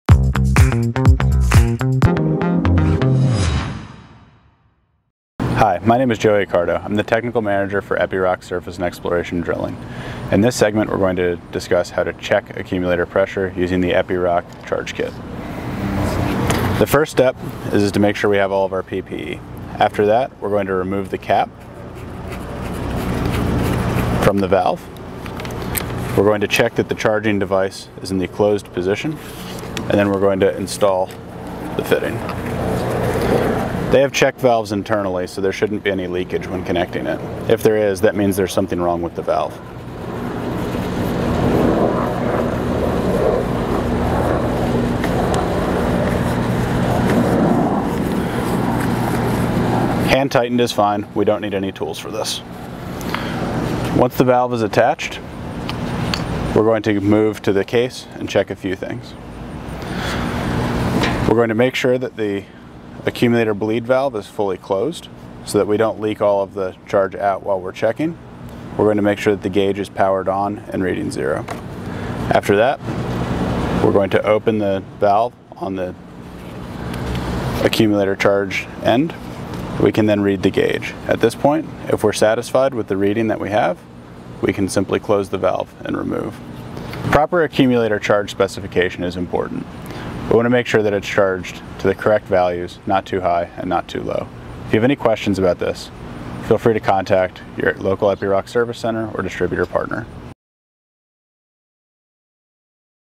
How to check the accumulator pressure | Epiroc rock drills and drifters
In this Epiroc 24/7 video, Joey Accardo, Technical Manager, guides viewers through the process of safely checking accumulator pressure using the Epiroc charge kit. Discover the necessary steps and precautions for maintaining optimal accumulator performance.

Applies to all Epiroc rock drills and drifters